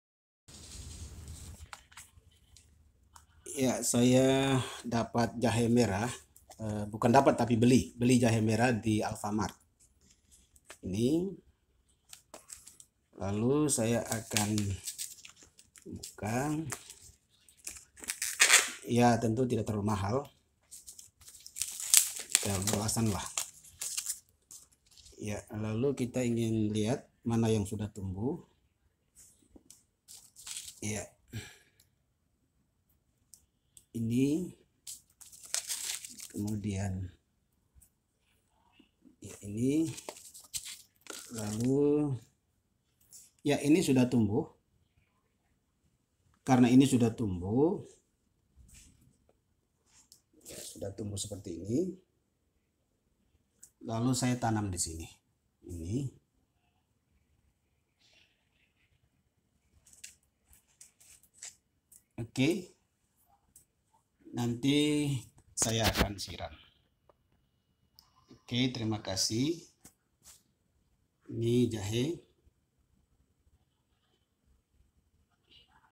Beli Jahe Merah untuk Ragam Manfaat
Jahe banyak khasiatnya terlebih lagi jahe merah. Saya membeli jahe merah di alfamaret. Ragam manfaat yang saya maksudkan disini yaitu: untuk keperluang konten dan kesehatan tubuh. Jahe sangat cocok untuk stamina, khususnya pria